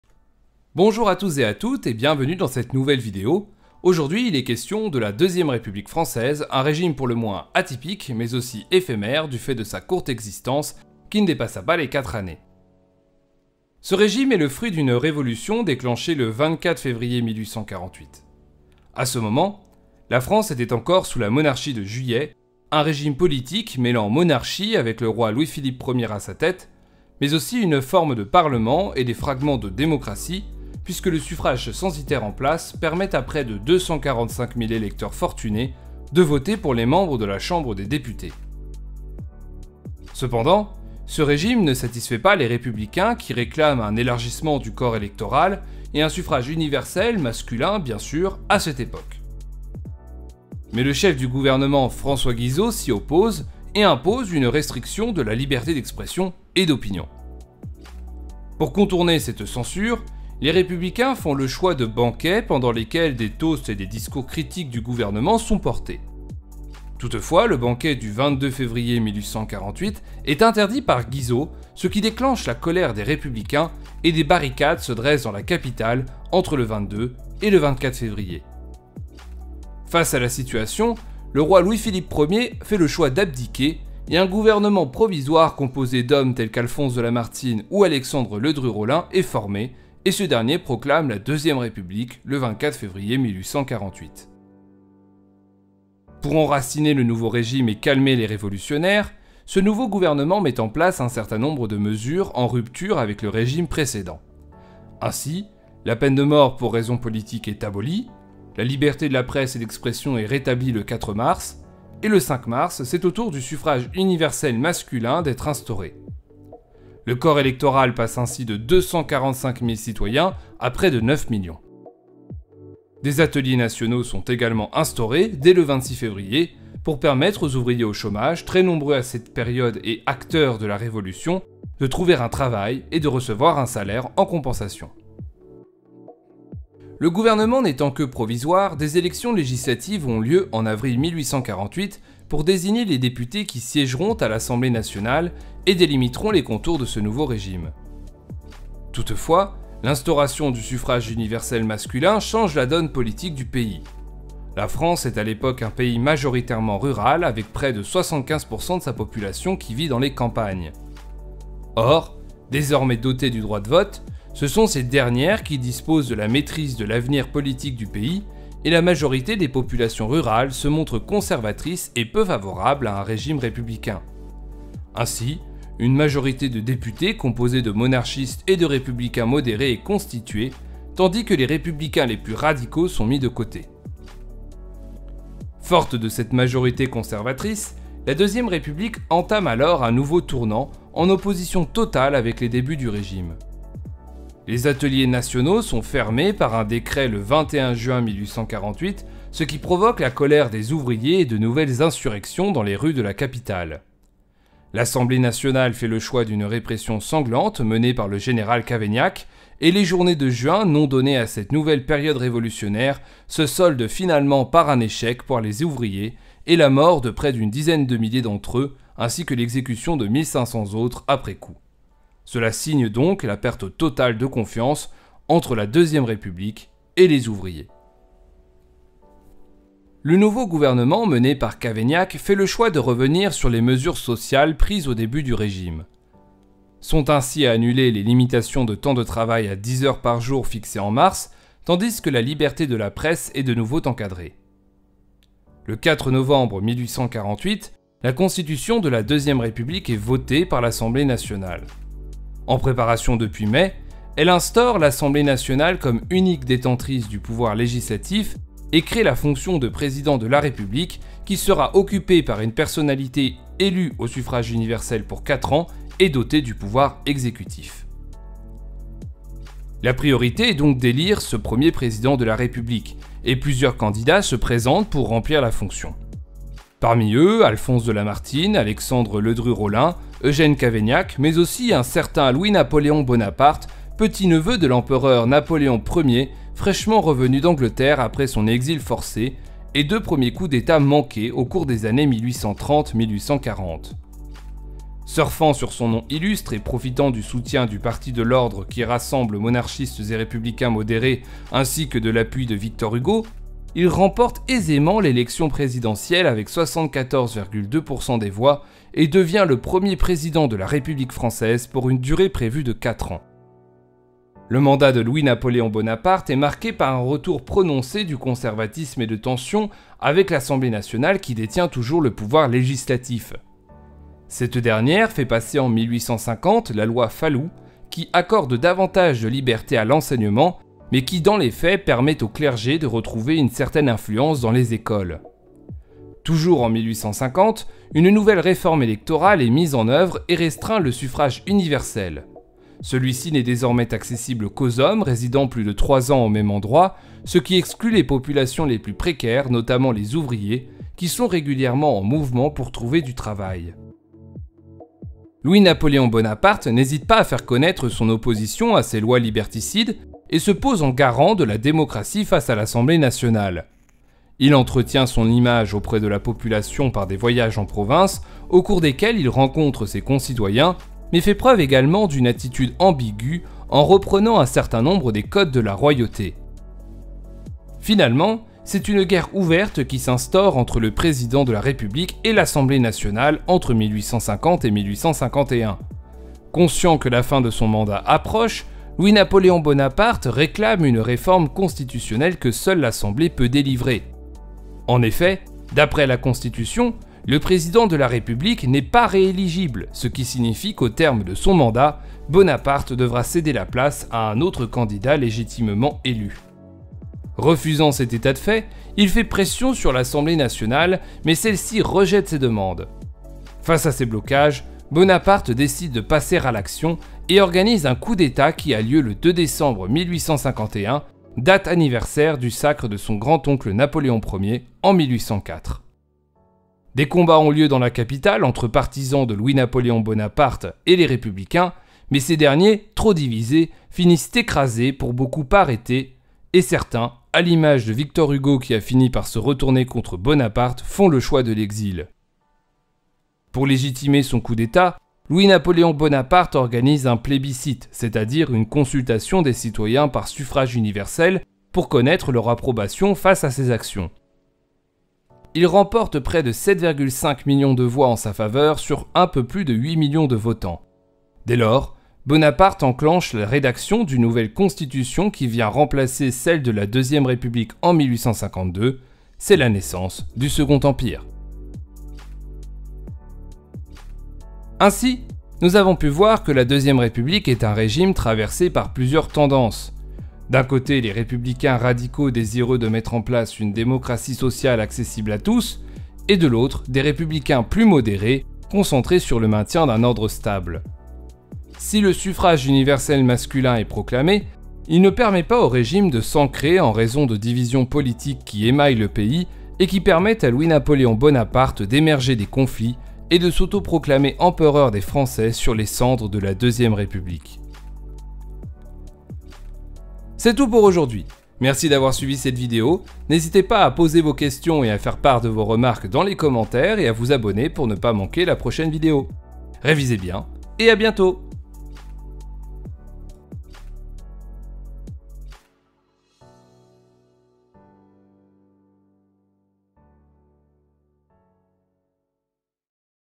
PREMIÈRES - LA DEUXIÈME RÉPUBLIQUE (1848-1852)
Nouvelle vidéo portant cette fois-ci exclusivement sur l'éphémère IIe République française en expliquant ses acteurs et événements importants pour éclairer la chronologie de cette période très dense. Bon visionnage !

Vidéo réalisée par P. Delaveau, enseignant d'histoire-géographie en région parisienne.